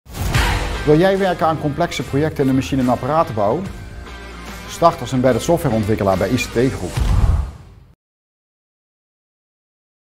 ICT GROUP vacature embedded software ontwikkelaar
Wil jij werken aan complexe projecten in de machine- en apparatenbouw? Start als embedded software ontwikkelaar bij ICT Group.

Creating a smarter world
ICT Group is een toonaangevende Europese leverancier van industriële technologische oplossingen.